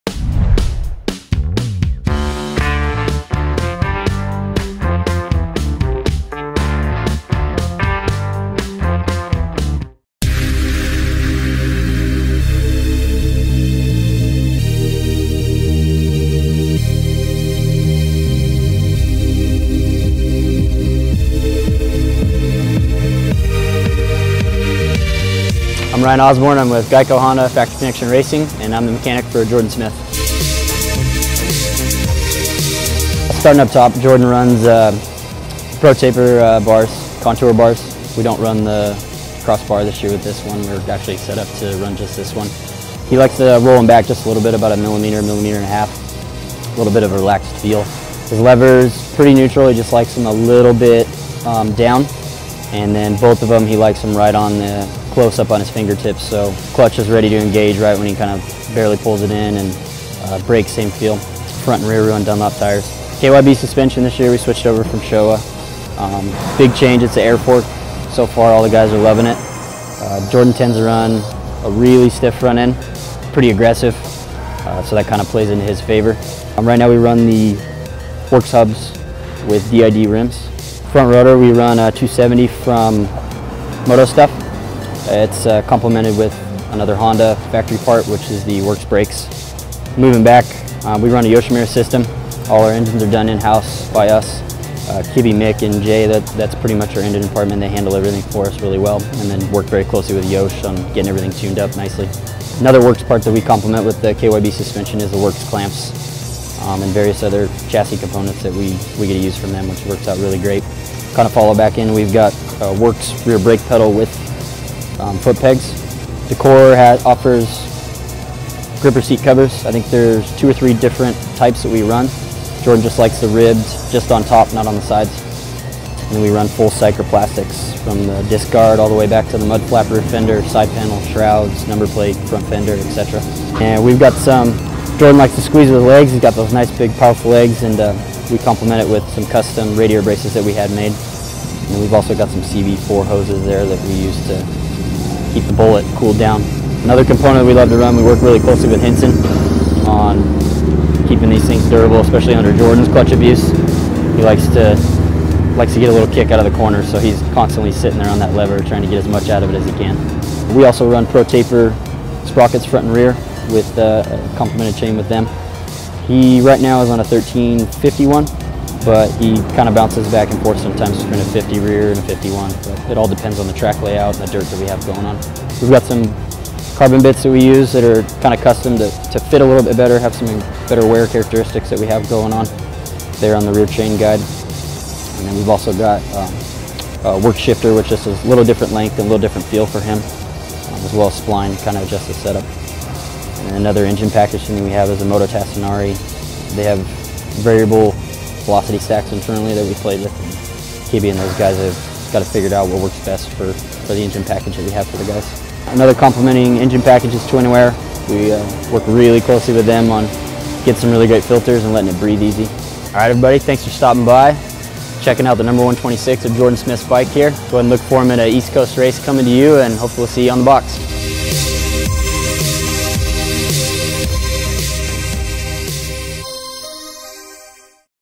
Inside Jordon Smith's Factory FC Geico Honda CRF250 - Inside the Pro's Bikes
For this episode of Inside the Pro's Bikes we take you on an inside look at Jordon Smith's Factory FC Honda. This bike is works from head to toe featuring works hubs, works shifter, Pro Taper Handle Bars, Hinson clutches, Dunlop tires, Yoshimura exhaust, and much more.